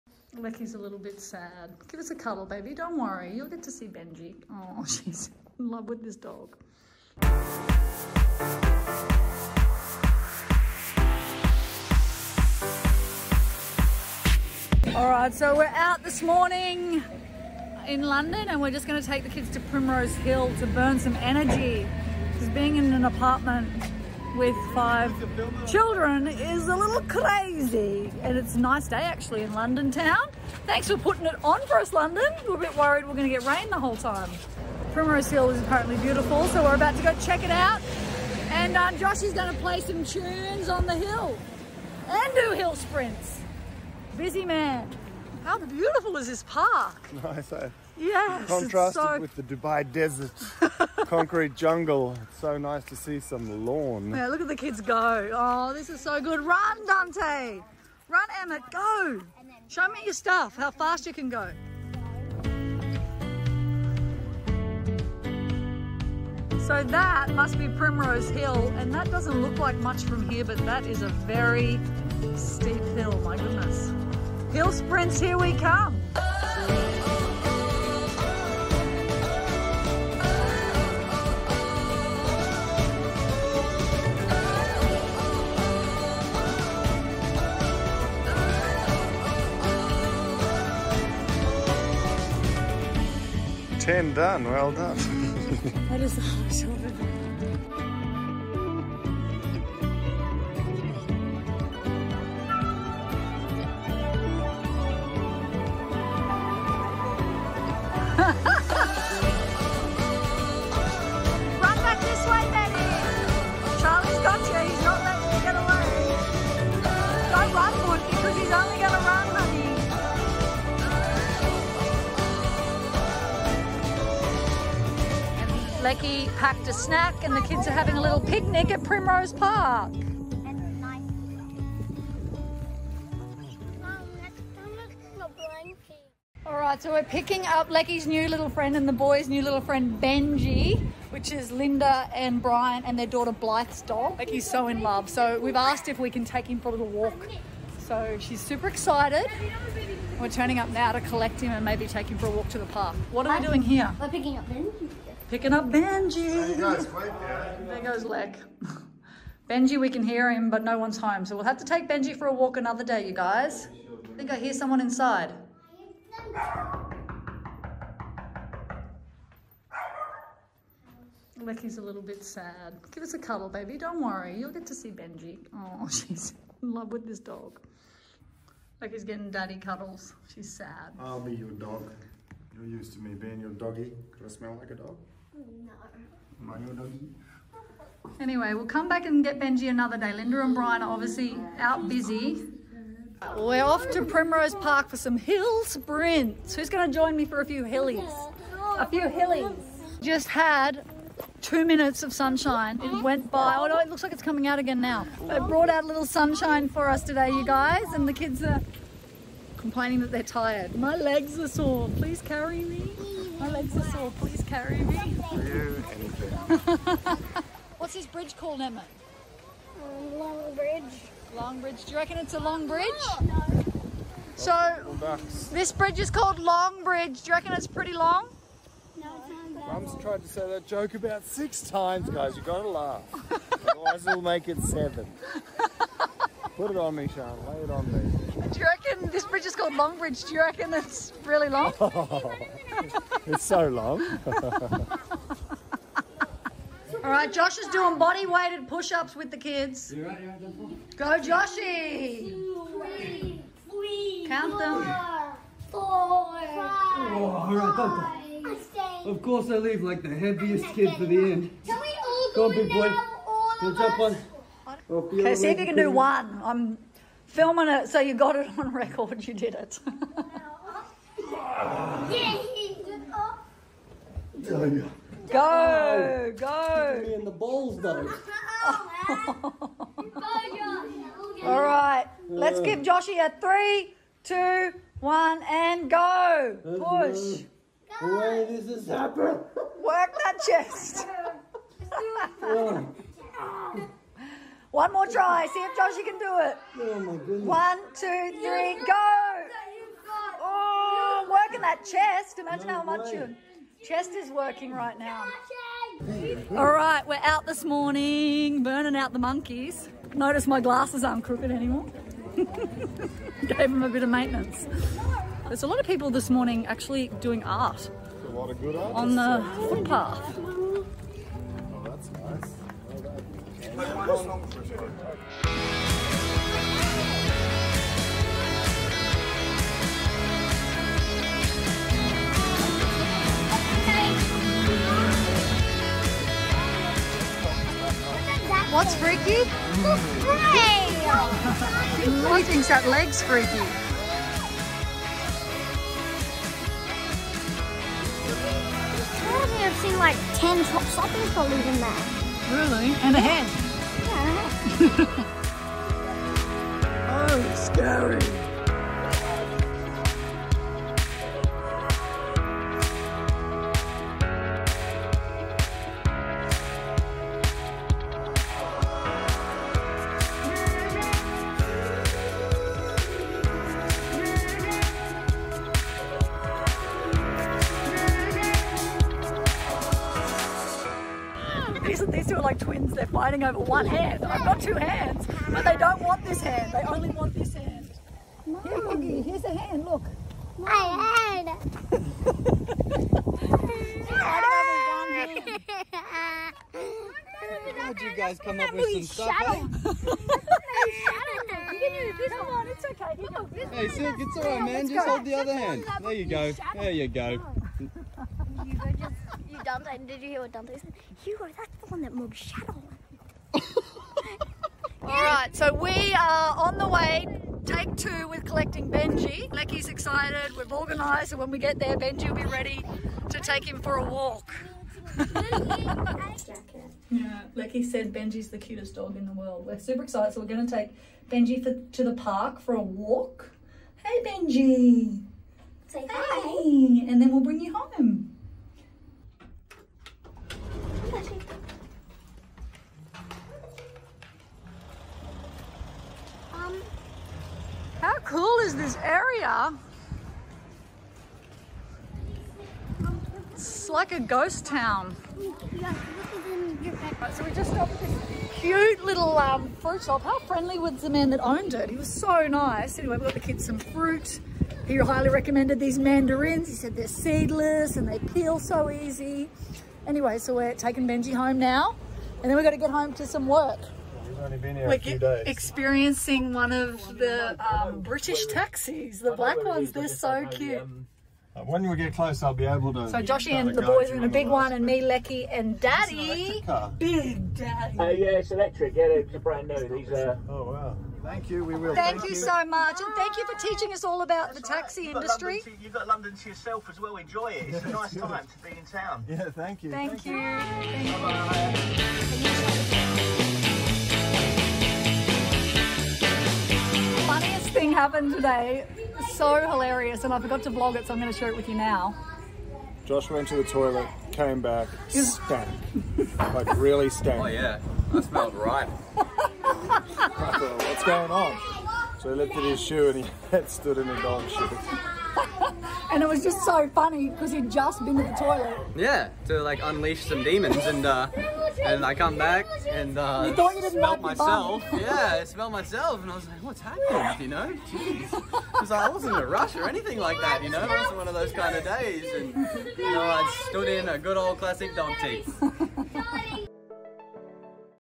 Episode 106: Exploring London
London gave us a bit of sunshine and we got to explore the town with the kids! Watch the video to see their favourite new friend 🐶